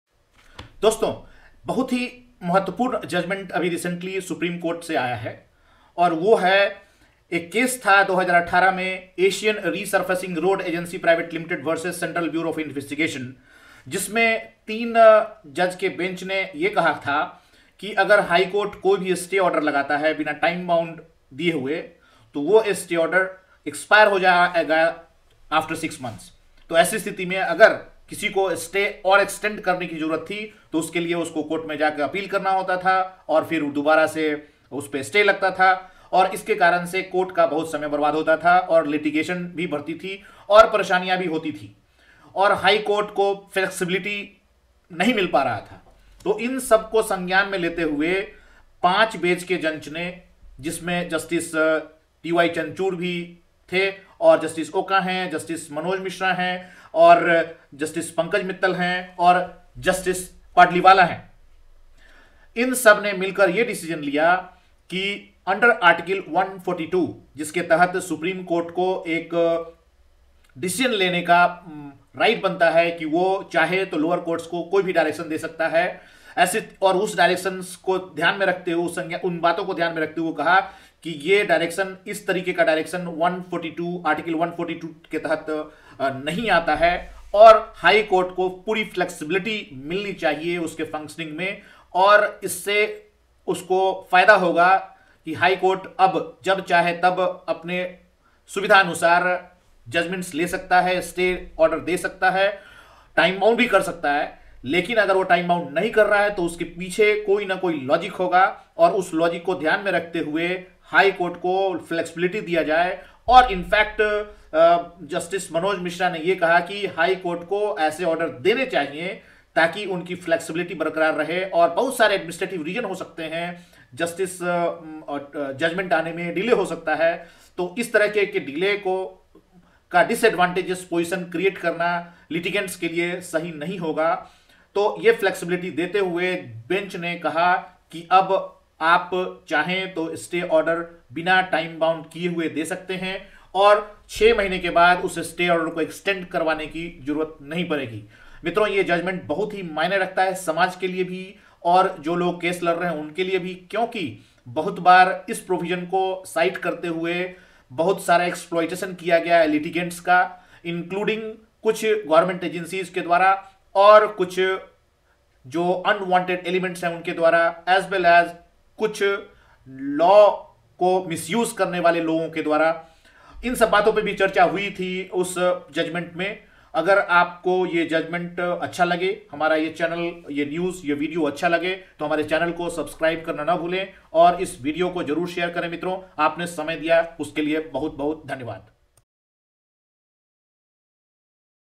Supreme Court Shocks Nation with STAY ORDERS Reversal in Asian Resurfacing Vs CBI Case
Friends, a very important judgment has recently come from the Supreme Court in the 2018 case of Asian Resurfacing Road Agency Private Limited vs. Central Bureau of Investigation. In this case, a three-judge bench had ruled that if the High Court gives a stay order without a time limit, the stay order would expire after six months. This meant that if someone needed to extend the stay, they had to go back to court and appeal, leading to increased litigation and wasted court time.

Recognizing these issues, a five-judge bench, including Justice DY Chandrachud, Justice Oka, Justice Manoj Mishra, Justice Pankaj Mittal, and Justice Pardiwala, reviewed this decision. They concluded that under Article 142, the Supreme Court has the authority to give directions to lower courts, but such directions related to the vacation of stay orders do not fall under this article. Therefore, the High Court should have full flexibility in its functioning.

This new ruling allows High Courts to issue stay orders without a time limit, providing greater administrative flexibility and reducing unnecessary delays for litigants. Justice Manoj Mishra emphasized that High Courts should regularly issue such orders to maintain their flexibility, considering various administrative reasons that might delay judgments.

This judgment is crucial for society and those fighting legal battles, as it prevents the exploitation of litigants by government agencies and other entities that misuse the law.

00:01 - Introduction: Important Supreme Court Judgment
00:29 - Overview of the 2018 Case and High Court Stay Orders
01:06 - Decision by a Bench of Five Judges
01:44 - Flexibility for High Courts Under Article 142
02:04 - Impact of the Judgment on High Court Orders
02:29 - Importance of the Judgment for Society and Litigants
02:52 - Conclusion: Encouragement to Subscribe and Share